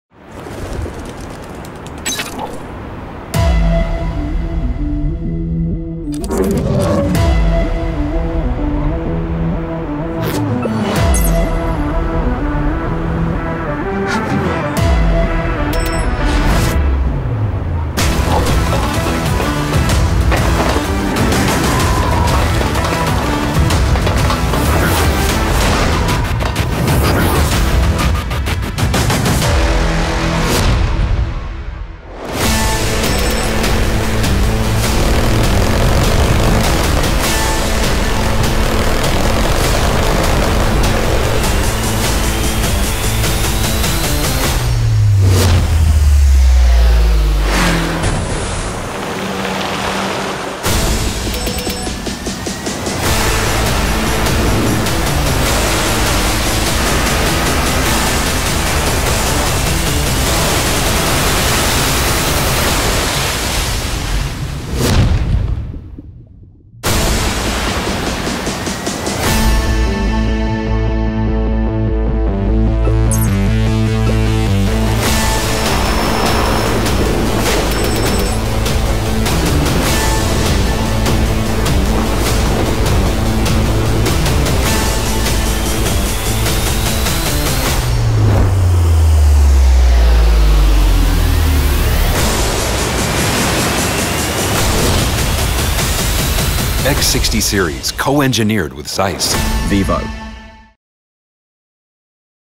vivoX60Series | Gimbal Stabilization 2.0
Get ready to capture the key movements of life with clarity and precision. PhotographyRedefined.
vivo Imaging lab tests the neXt. Watch the all new vivoX60Series co-engineered with ZEISS pass extreme tests as Gimbal Stabilization 2.0 redefines motion photography.